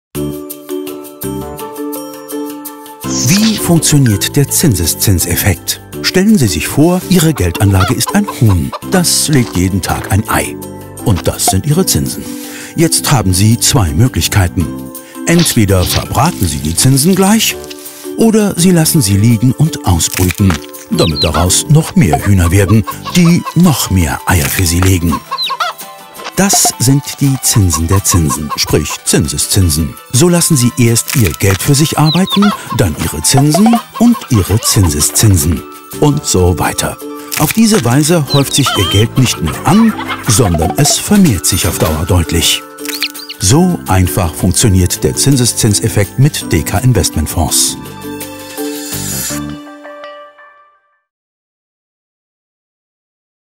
Dekabank Zinseszins
I wrote and produced the music for this animated film.